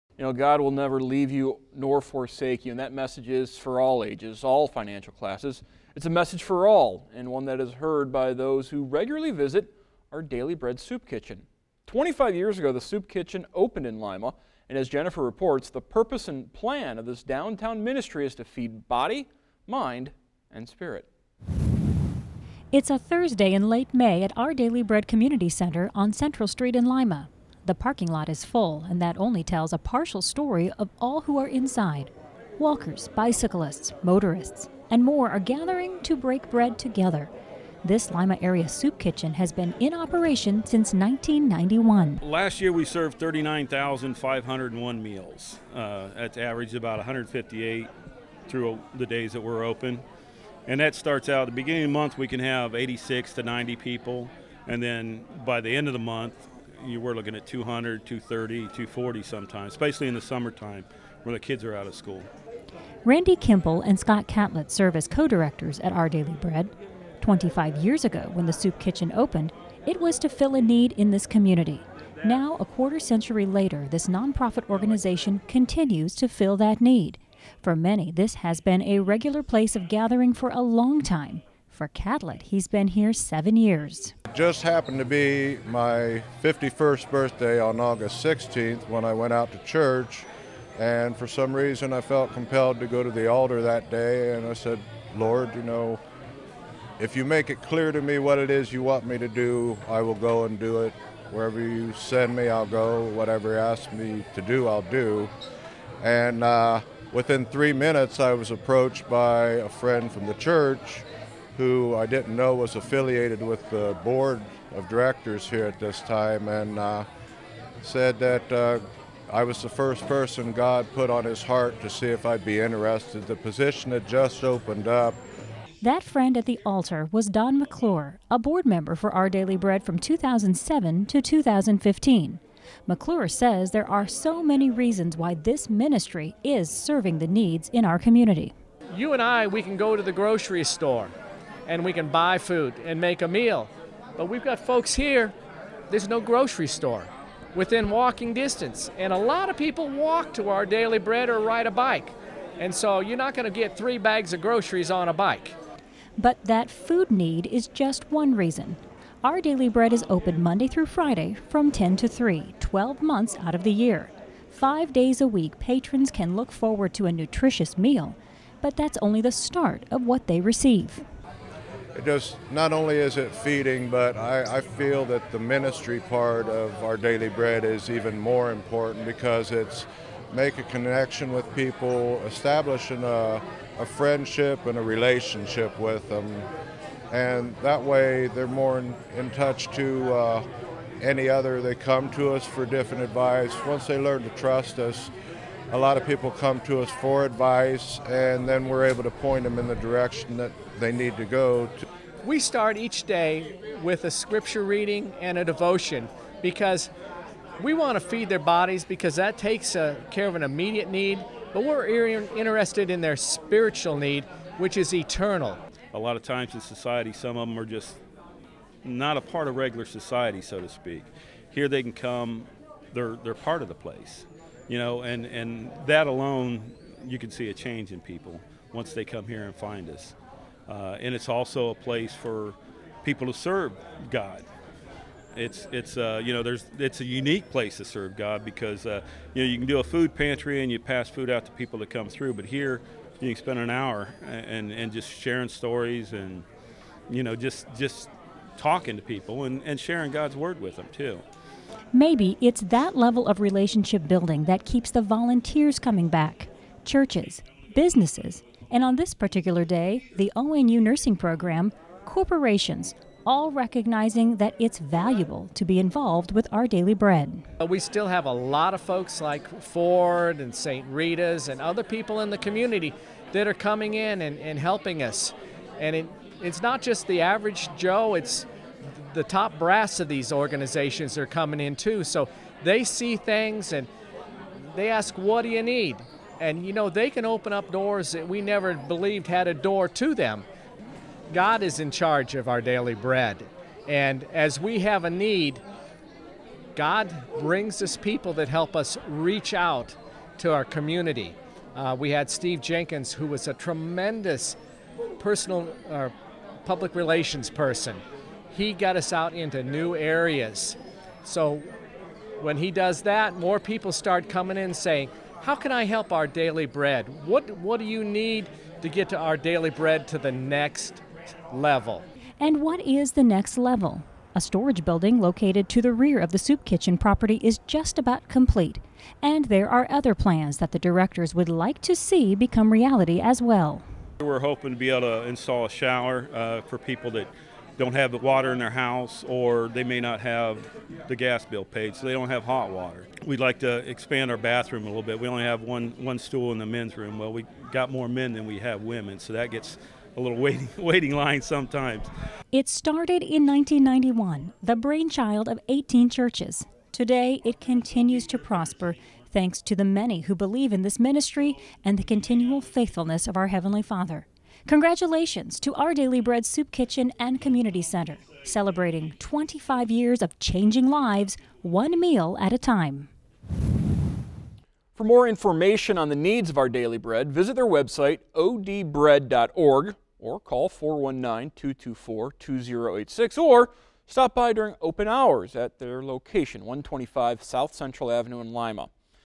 Our Daily Bread 25th anniversary
Our Daily Bread Soup Kitchen and Community Center is celebrating it's 25th year in Lima, OH. Hundreds of thousands of meals since the organization opened its doors, the organizers say the mission to provide food: body, mind, and spirit, is stronger than ever.